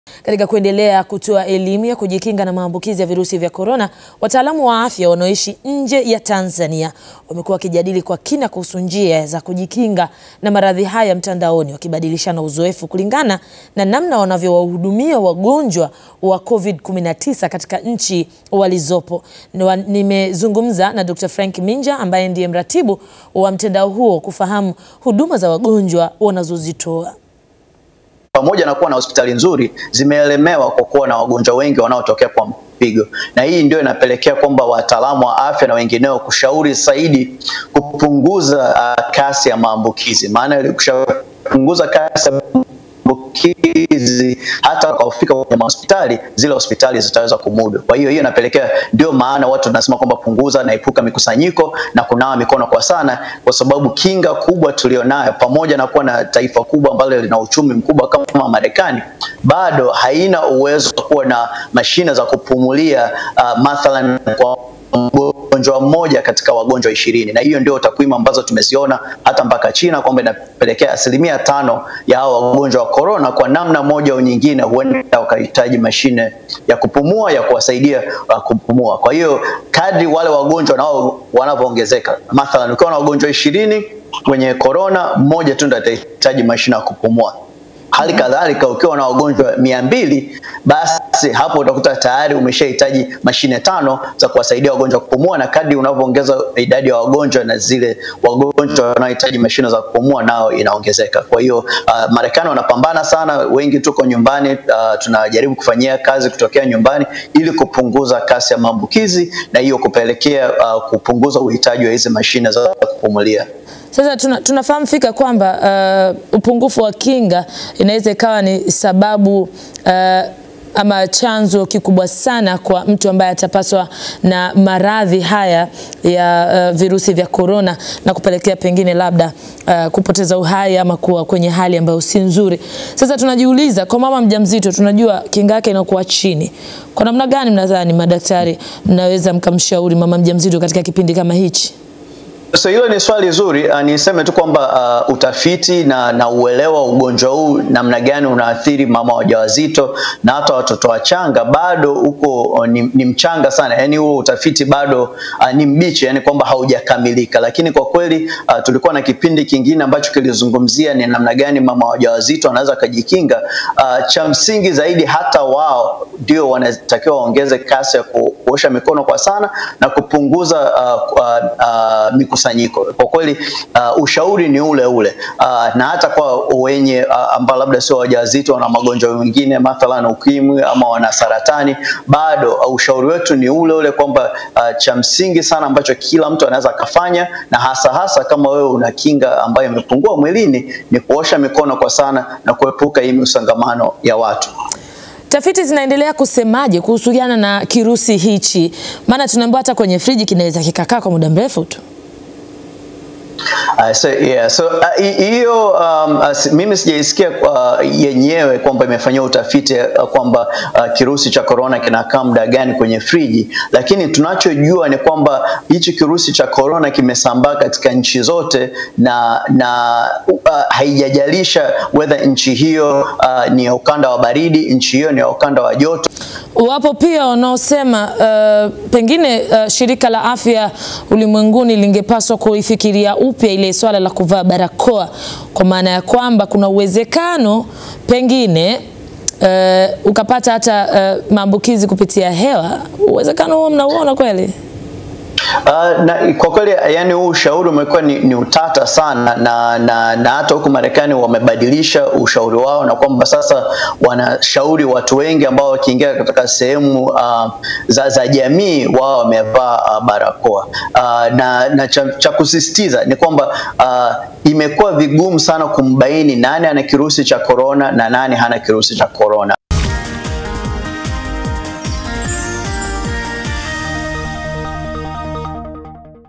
Wataalam wa afya wanaoishi nje ya Tanzania wanavyopambana na Corona
Kuendeleza elimu ya kujikinga na maambukizi ya virusi vya Corona, wataalam wa Afya wanaoishi nje ya Tanzania wamekuwa wakijadili kwa kina kuhusu njia ya kujikinga na maradhi haya mtandaoni wakibadilishana uzoefu kulingana na namna wanavyowahudumia wagonjwa katika nchi walizopo.

Nimezungumza na Dkt. Frank Minja ambaye ndiye mratibu wa mtandao huo kufahamu huduma za wagonjwa wanazotoa.